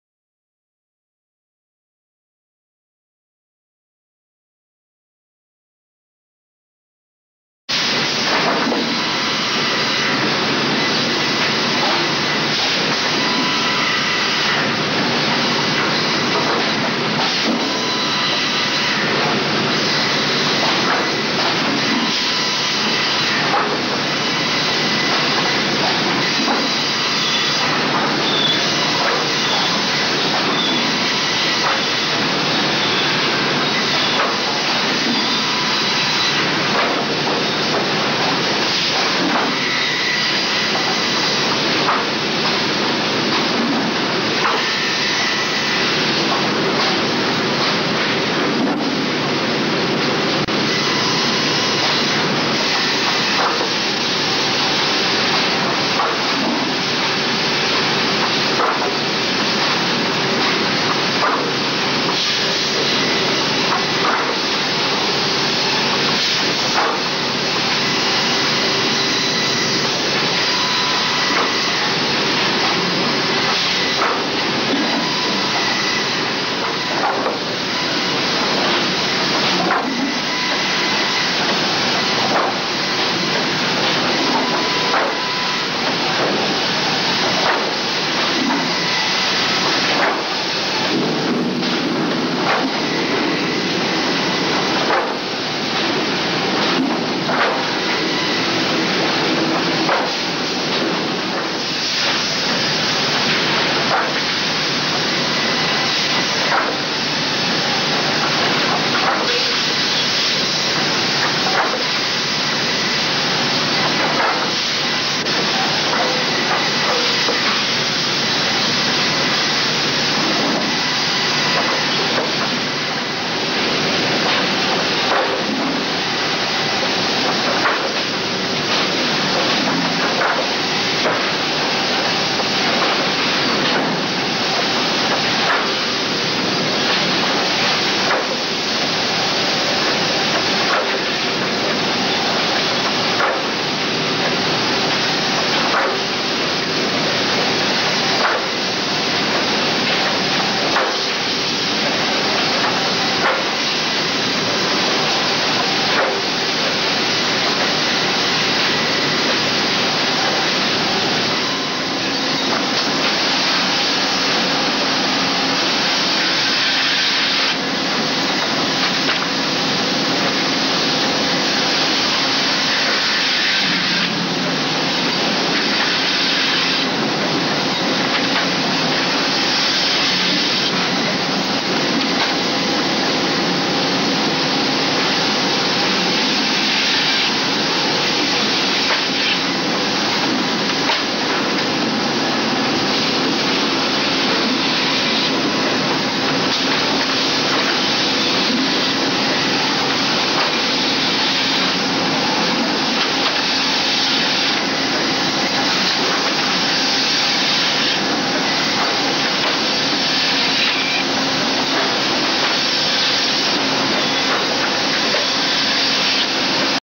Circular Line Bar Auto Infeed
The Pendu Circular Line Bar is a multi-saw, double arbor system that performs three different functions — automatically self centers split edged boards or cants up to 8" cross section using one to three saws, resaws single boards thicker than 3/8" and cants up to 8" x 8" in cross section, or reclaim from one to three boards per pass from edged heavy slabs typical of those produced on scragg mills.